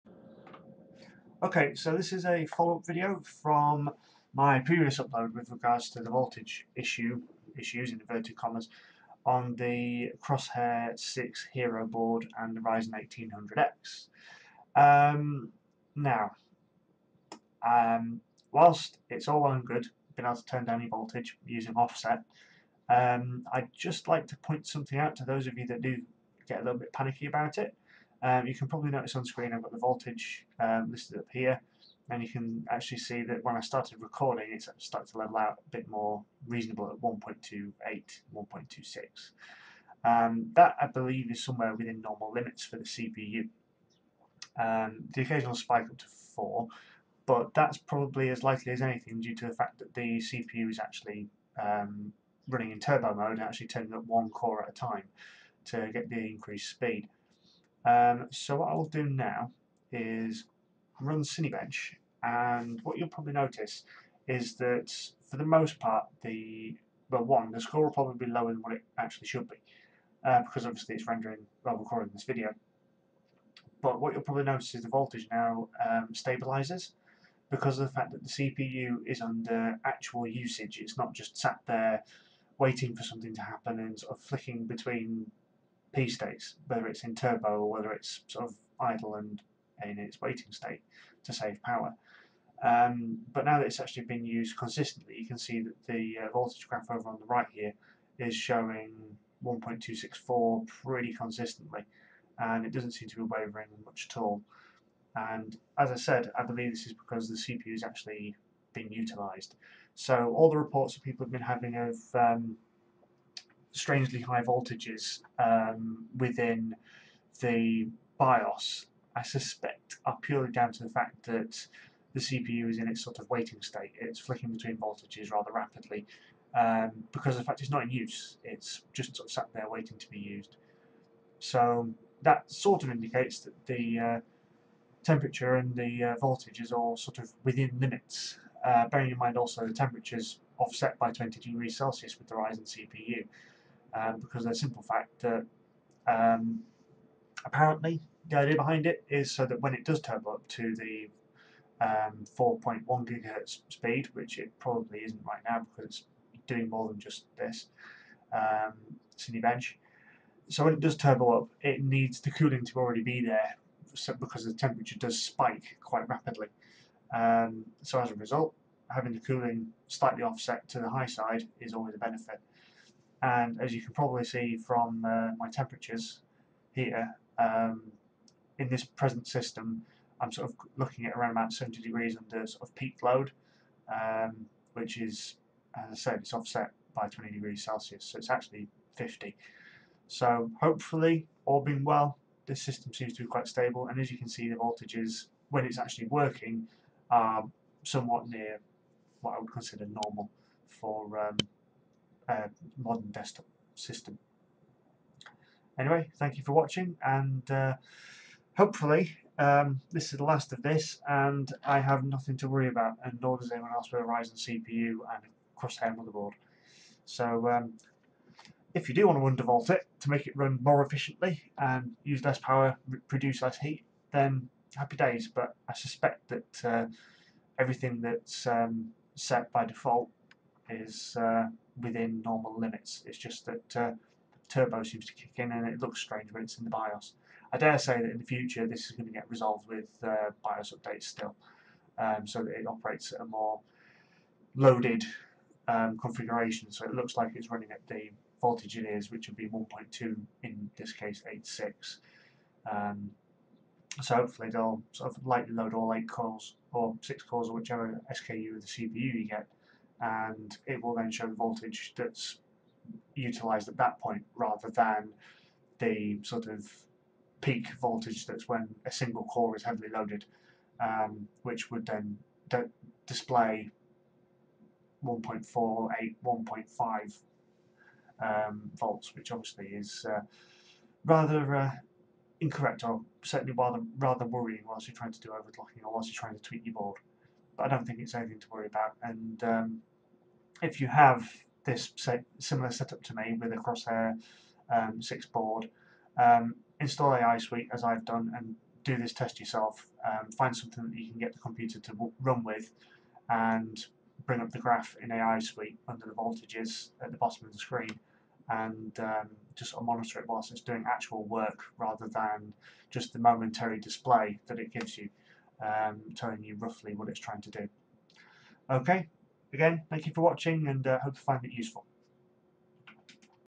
Crosshair VI Hero and Ryzen R7 1800X Voltage - The follow up!
So, after another rather rudimentary test, and some graphs, it seems the voltage listed in the UEFI is only whilst a single core is loaded up, the "turbo" voltage is you will, however using a graph and putting a load across the CPU tells a much clearer and less worrying story!